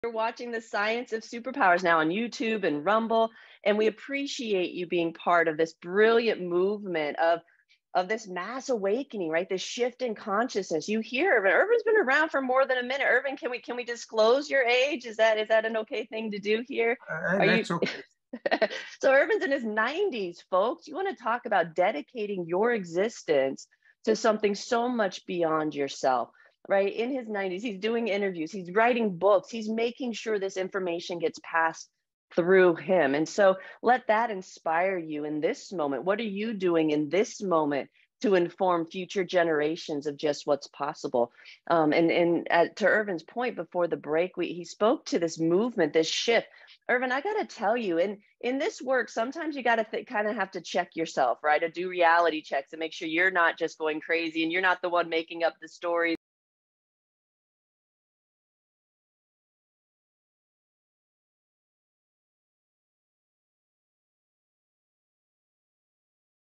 Get Inspired by Ervin Laszlo
As part of our Men of Co-Creation celebration, Ervin Laszlo joins Tonya Dawn Recla again as they talk about the futre of humanity. Ervin is an amazing being who has dedicated his life to supporting humanity during this massive transition. In this episode, he and Tonya share steps you can take right now to reinvigorate your zest for life.

Watch now to remember that we are incredibly creative and powerful when we set our sights on goals bigger than ourselves.